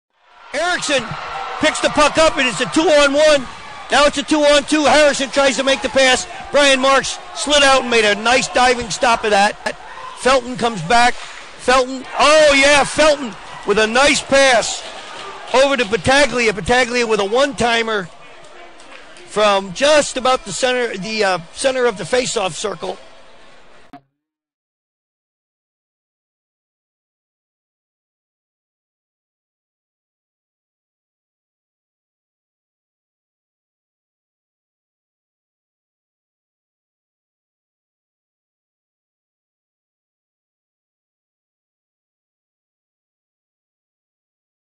Brian Marks Turn Defense Into Offense in Watertown
Marks' defensive play on a 2 on 1 against send the puck the other way and Anthony Battaglia finishes the play with a goal off a pass from Jesse Felten.